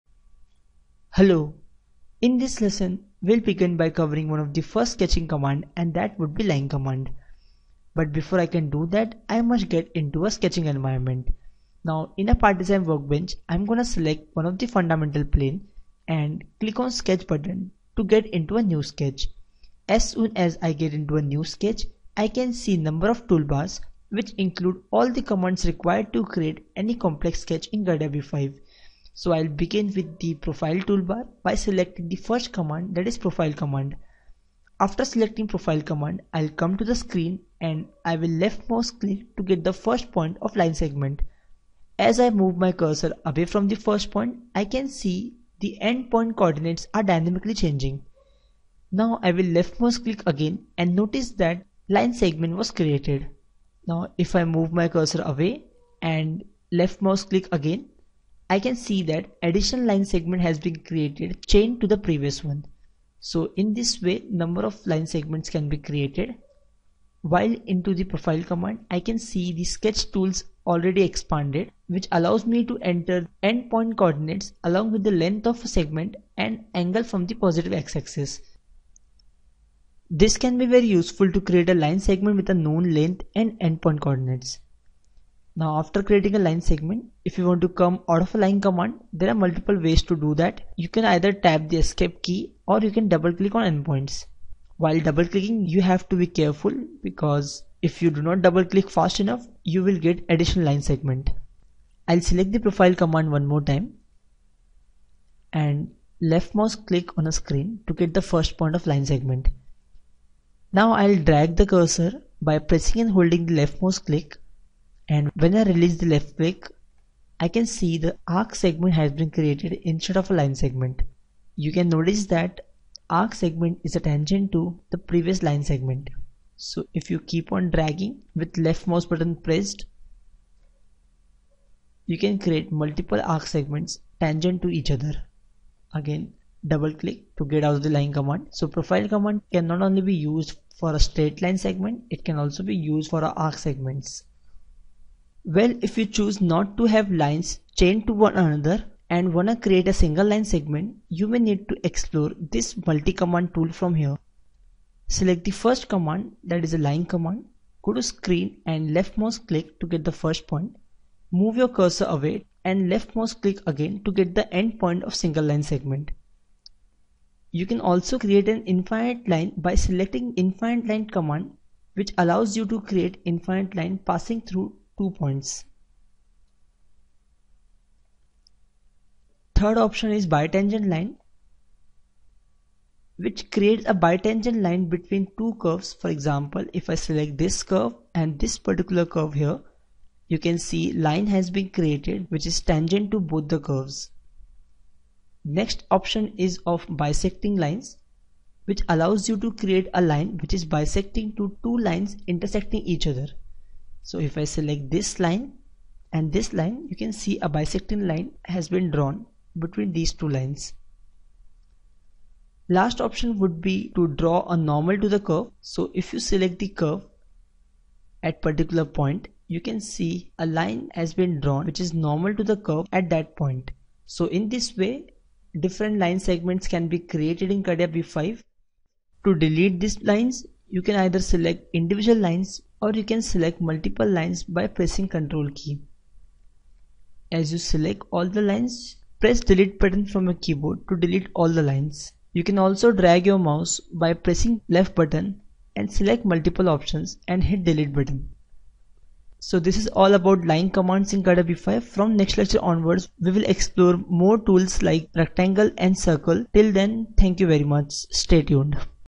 Catia v5 2015 full series Tutorials | Using Line Commands_Lesson 2.2
Introduction to learning Catia v5 2015 by Abhishek Agrawal

Lesson 2.2 : Using profile and Line commands

In this tutorial,I have explained all types of line command in CATIA v5 sketching environment as follows:
1) Profile command (for line and arc segment)
2) Line command
3) Infinite Line command
4) Bi Tangent Line command
5) Bi Secting Line Command
6) Line normal to curve